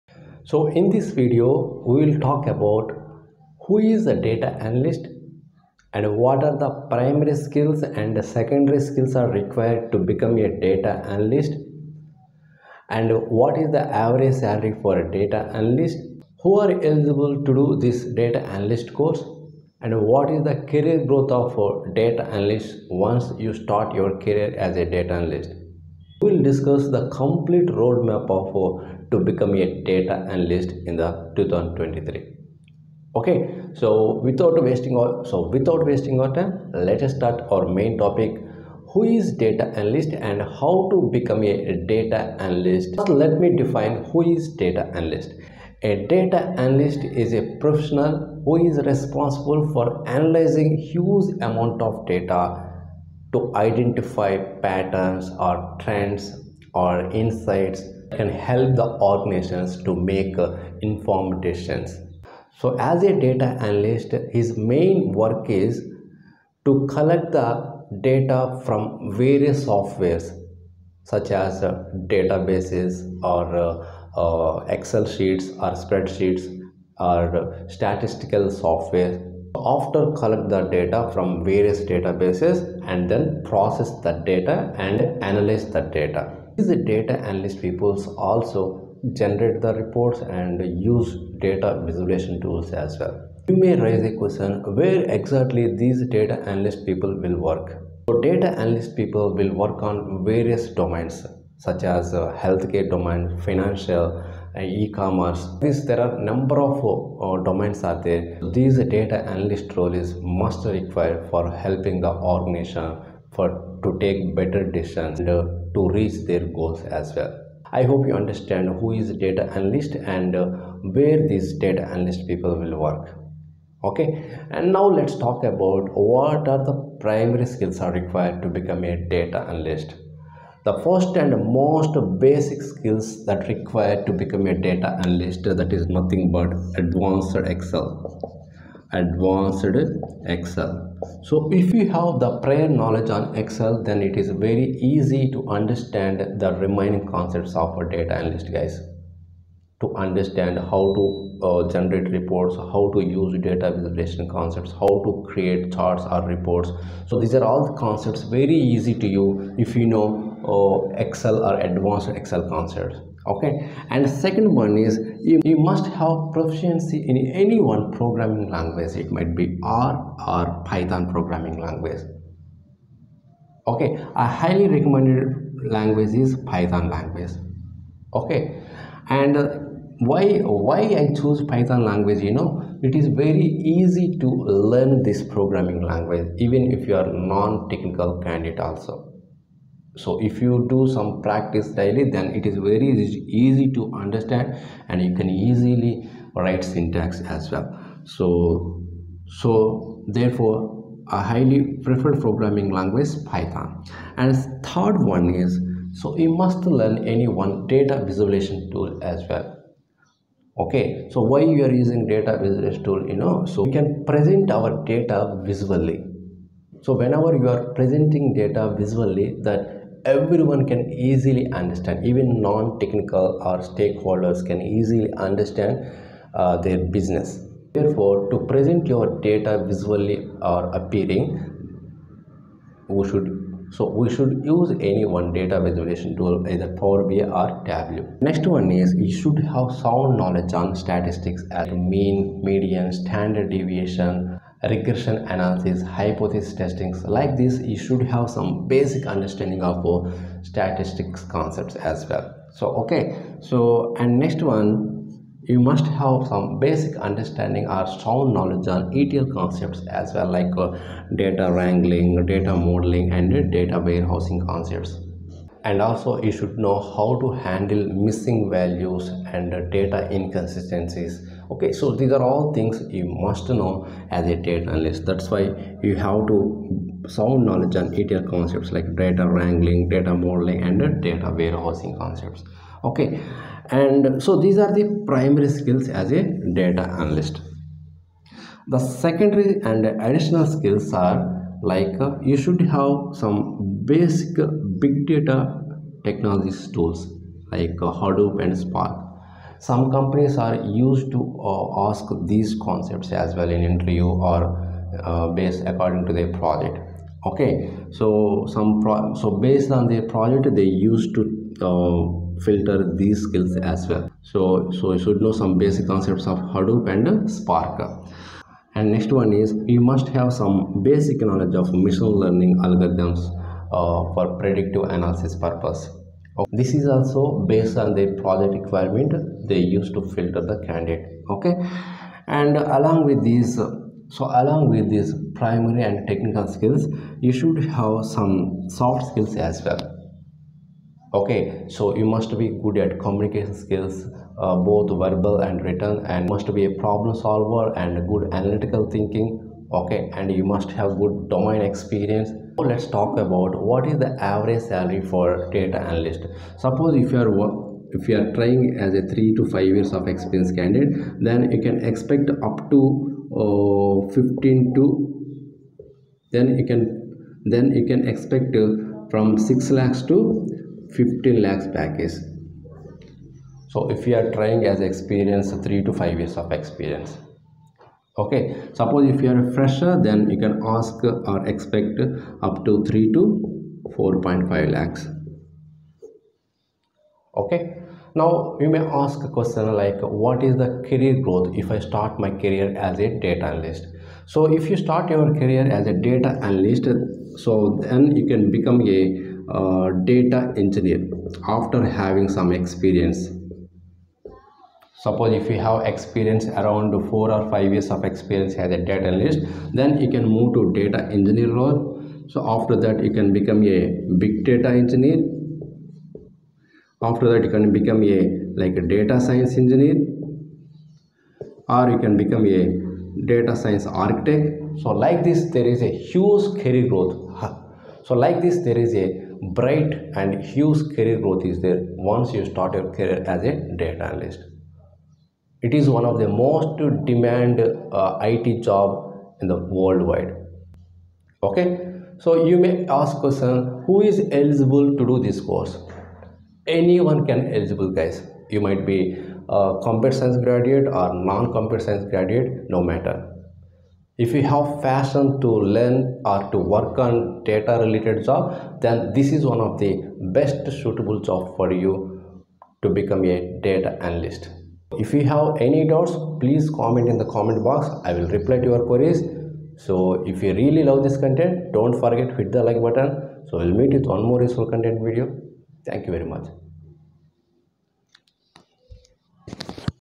How to Become a Data Analyst in 2023
who is data analyst and how to become data analyst and what are the primary skills and secondary skills required to become a data analyst and what is the average salary for data analyst and career growth of a data analyst.